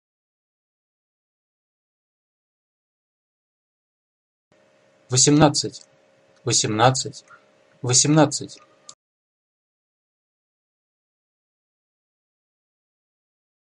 Numbers in Russian. 18 (eighteen). Numerals 10-19. How to pronounce and spell correctly.
EIGHTEEN (18) / ВОСЕМНАДЦАТЬ / VASEMNATSAT`

Numbers in Russian from 10 to 19. Transliteration from Cyrillic to Latin. Correct Russian pronunciation from a native speaker.